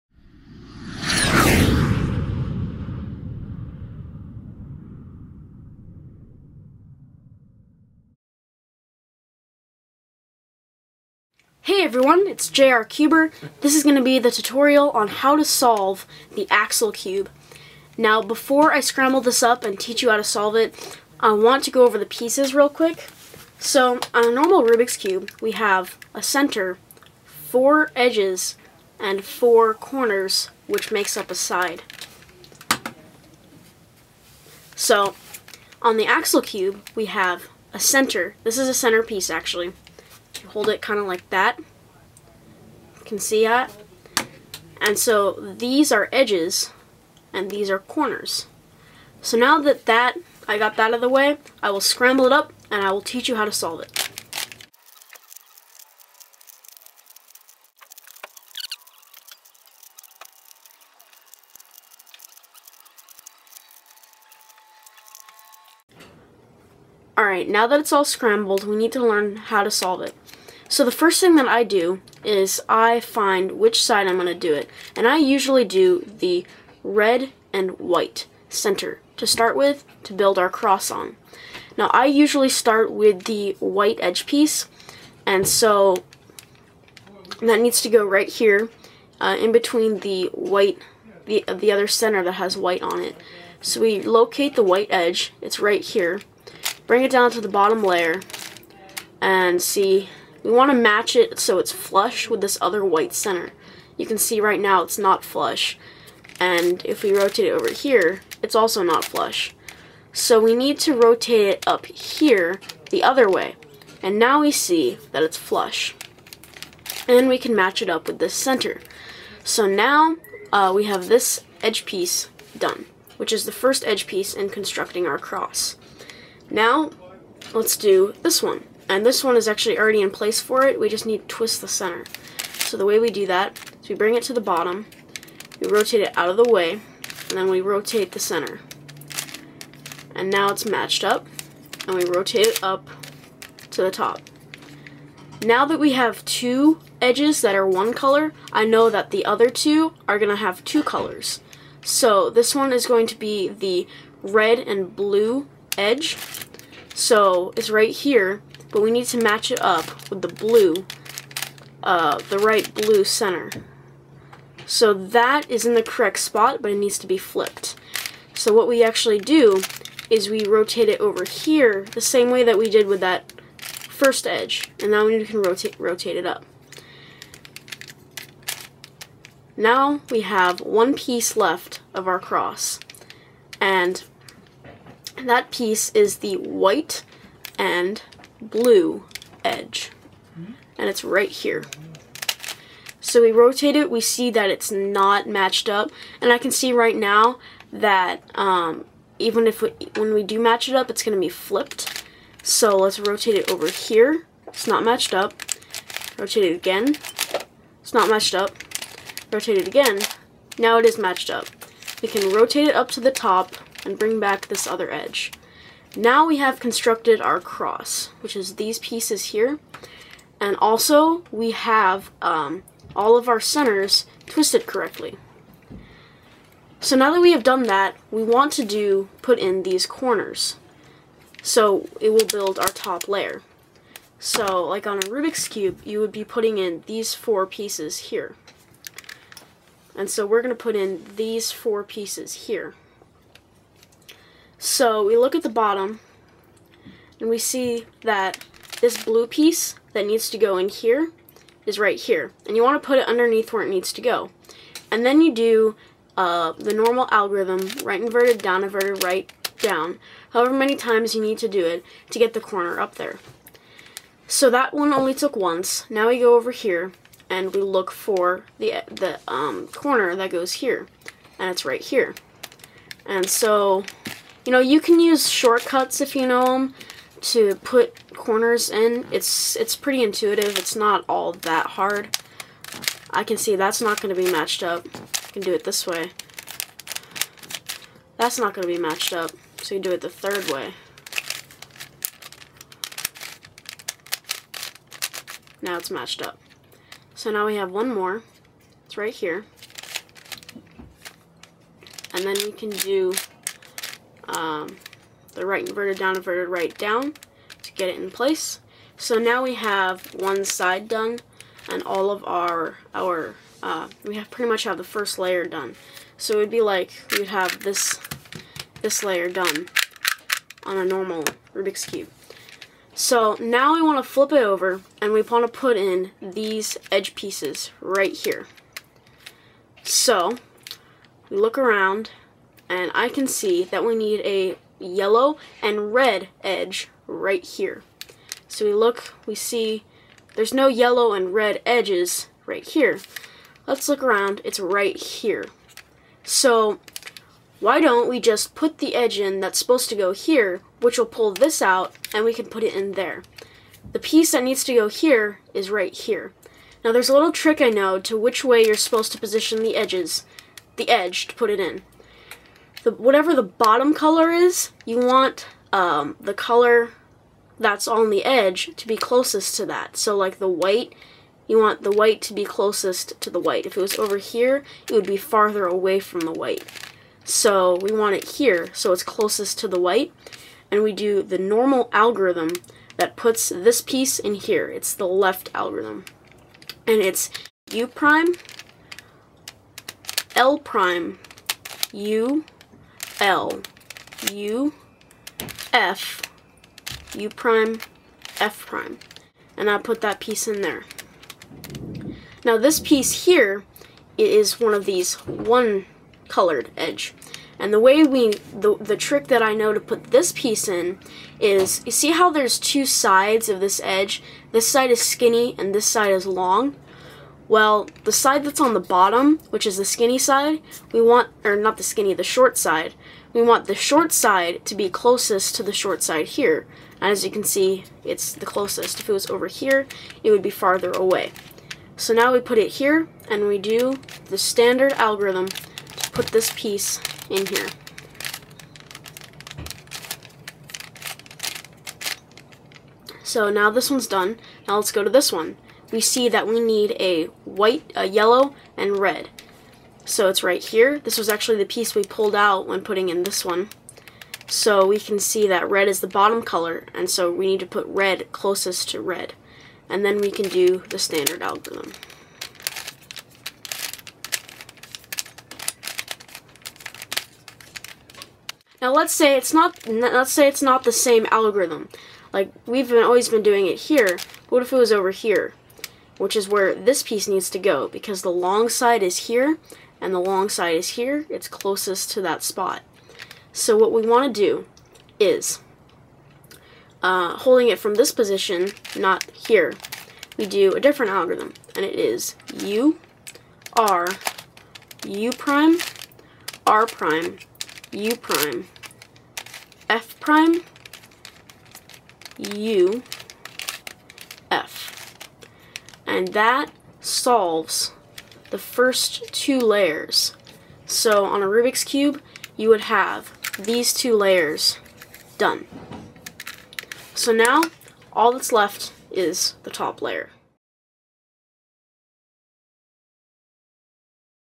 How To Solve The Axel or Axis Cube - Part 1
This is the tutorial on how to solve the Axel or Axis cube! Before you can begin solving the Axel cube, you must have good experience with the beginners method of the 3x3. Happy cubing!

Algorithms:

1. R' D' R D (corner orientation)

2. U' L' U L U F U' F'   (Left algorithm)

3. U R U' R' U' F' U F   (Right algorithm)

4. F R U R' U' F'    (Top layer cross algorithm)

5. R U R' U R U2 R'    (Top layer cross edge switching algorithm)

6. U R U' L' U R' U' L   (Top layer corner switching)